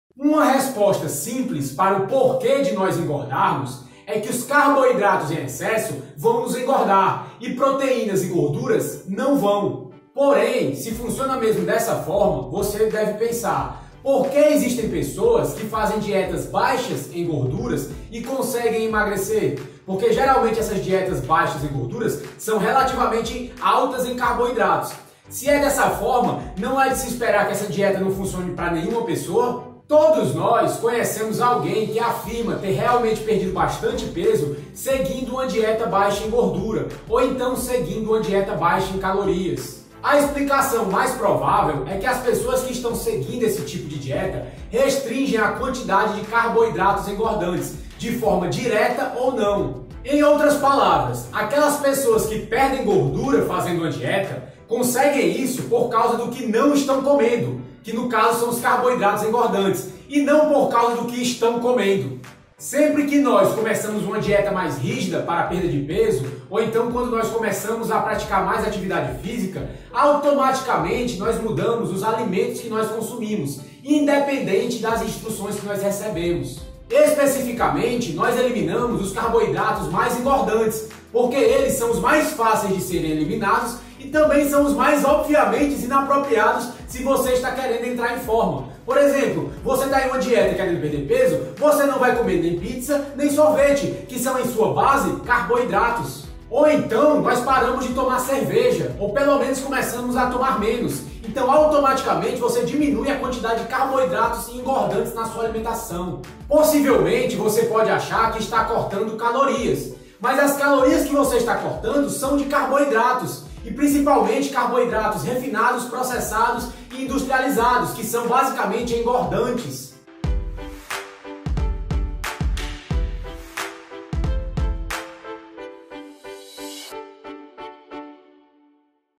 não caia na MENTIRA DE DIETAS com baixa gordura e caloria
Você acredita que dietas com baixa gordura ou com diminuição de calorias funcionam?

Todo mundo conhece alguém que afirma que realmente conseguiu perder bastante peso com um desses tipos de dietas, porém o que fez eles emagrecerem não tem nada a ver com diminuir gorduras ou calorias.

Agendamentos de consultas ONLINE pelo wpp: (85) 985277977